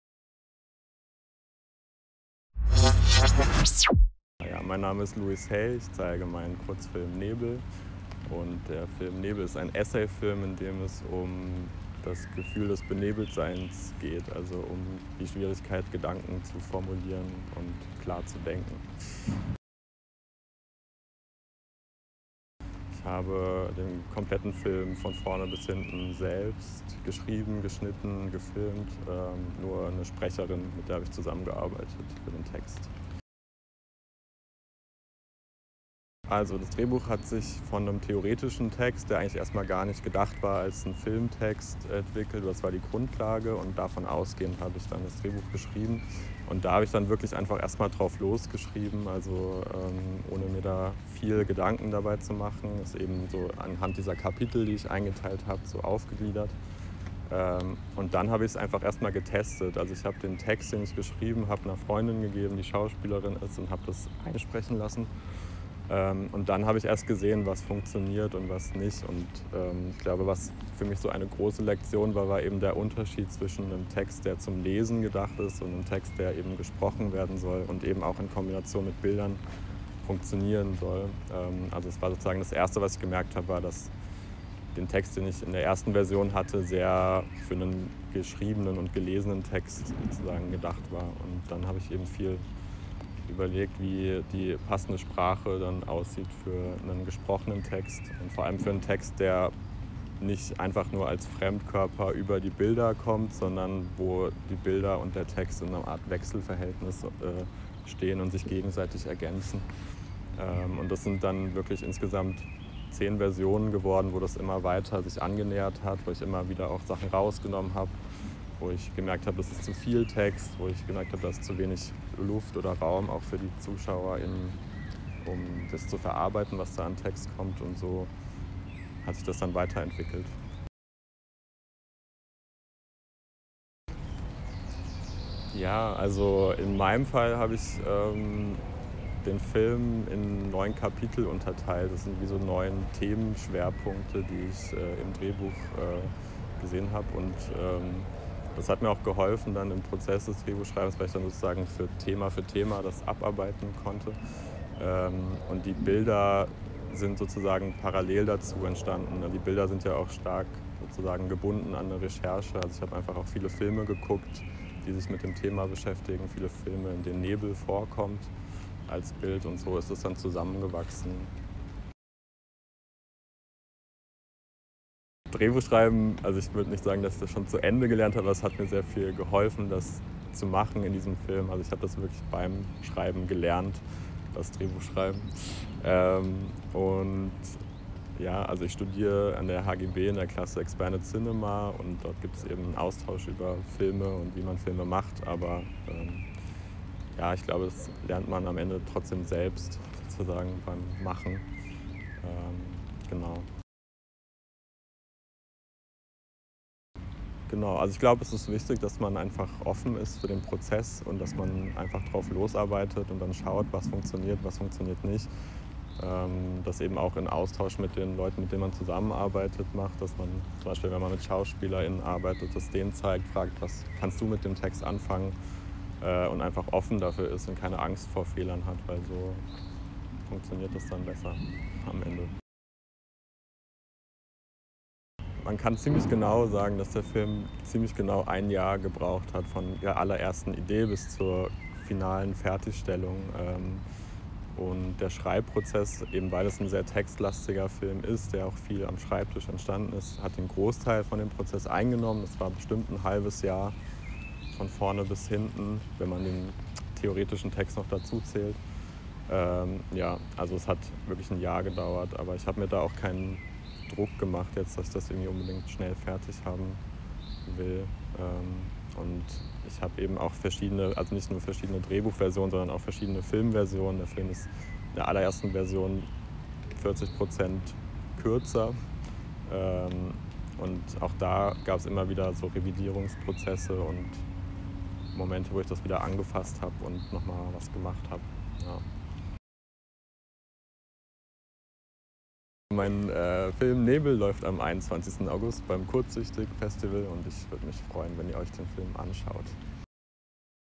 KURZgesagt - Wie schreibt man ein Drehbuch?
Im Gespräch mit Kurzsuechtig erzählt Louis Hay, wie er das Drehbuch zu seinem Kurzfilm "Nebel" geschrieben hat. Sein Kurzfilm läuft am 21.08. bei Kurzsuechtig.